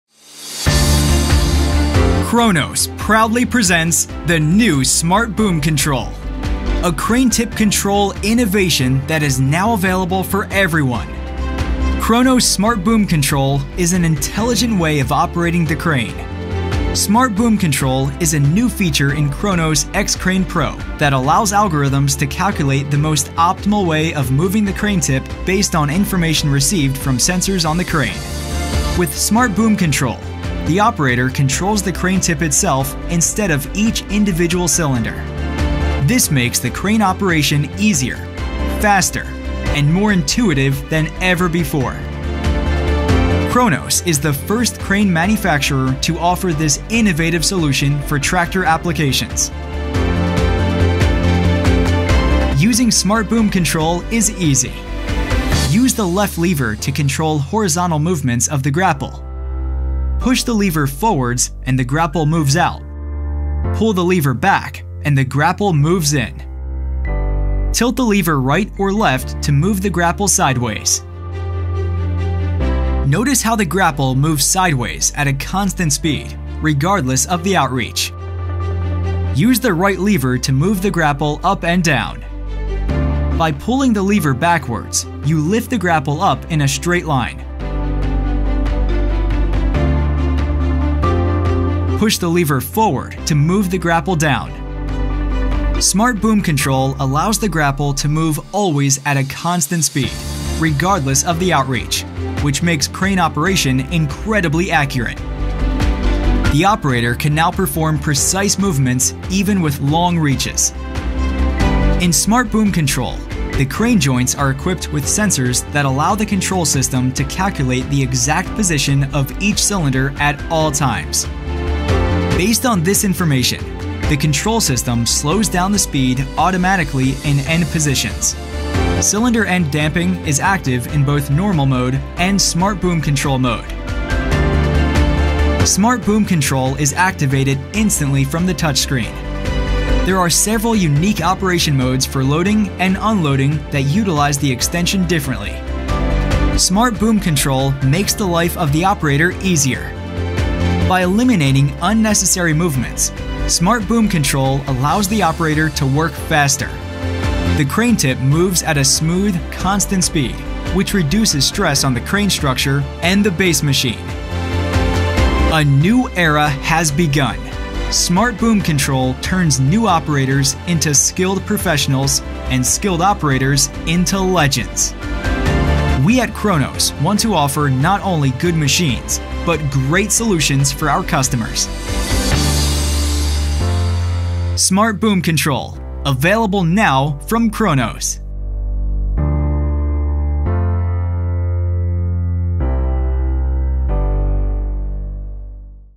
Kronos Smart Boom Control (SBC) for forest crane
Kronos Smart Boom Control (SBC) controls the tip of the forestry crane automatically. Don't worry about each individual hydraulic cylinder, but steer the grapple loader exactly where you want it.